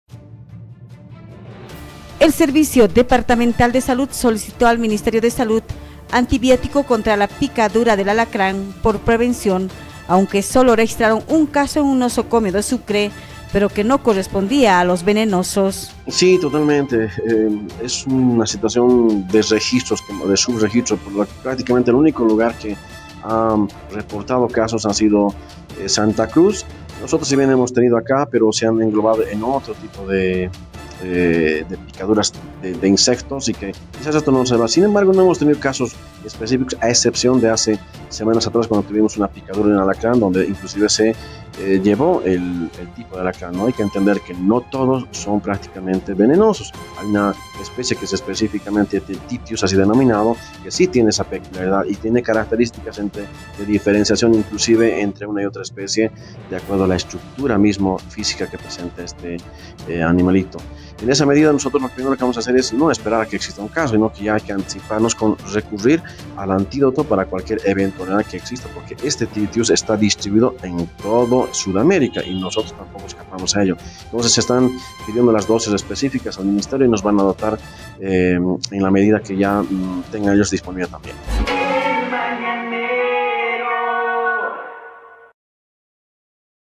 SOLICITARON SUERO ANTIBIÓTICO CONTRA PICADURA DE ALACRÁN
El Sedes solicitó al Ministerio de Salud en antibiótico contra la picadura del alacrán por prevención, aunque solo registraron 1 caso en un nosocomio de Sucre, pero que no correspondía a los venenosos.